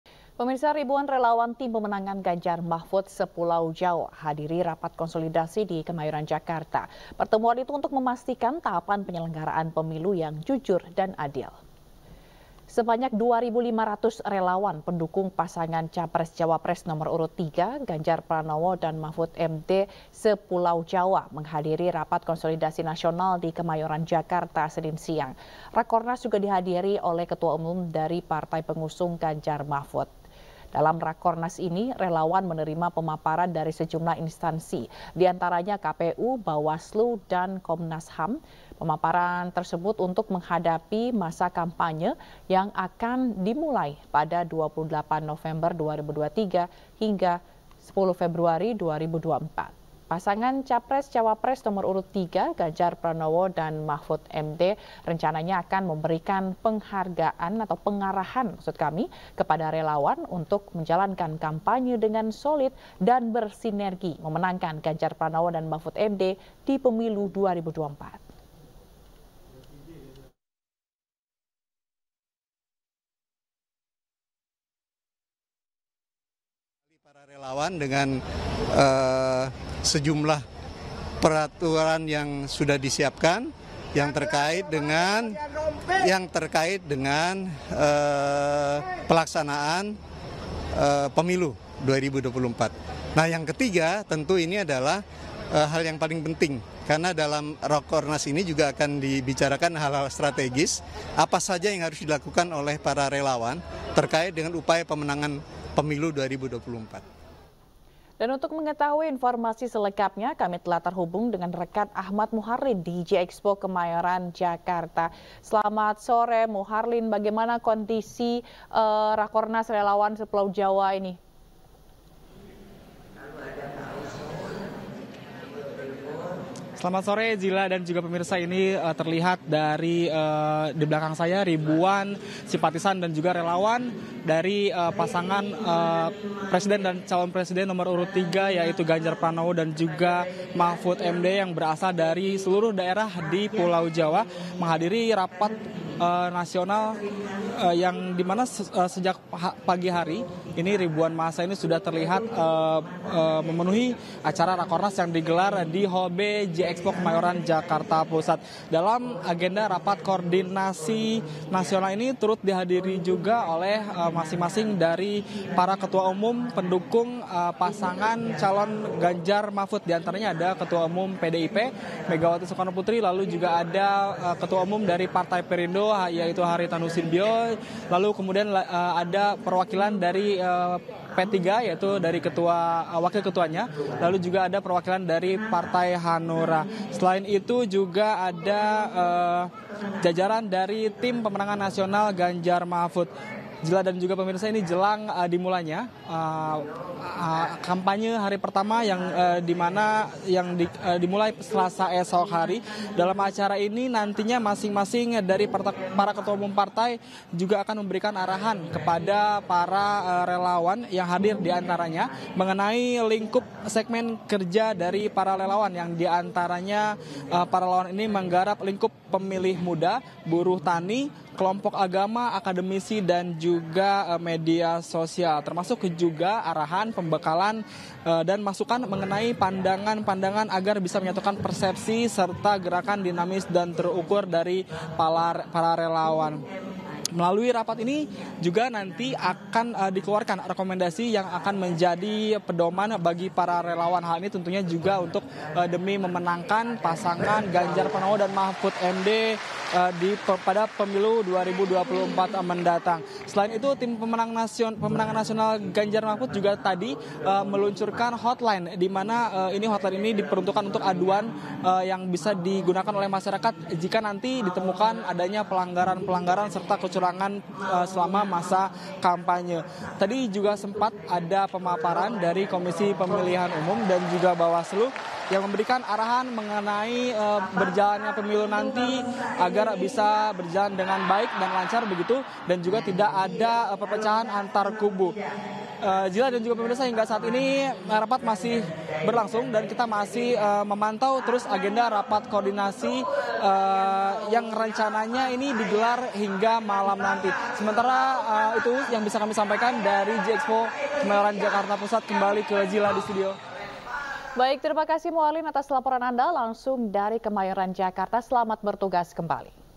Ribuan Relawan Ganjar-Mahfud Sepulau Jawa Hadiri Rakornas di Kemayoran
Ribuan relawan tim pemenangan Ganjar-Mahfud sepulau jawa hadiri rapat konsolidasi di Kemayoran, Jakarta. Rakornas juga dihadiri oleh ketua umum partai pengusung Ganjar-Mahfud. Dalam acara ini, relawan mendapat pemaparan pada masa kampanye.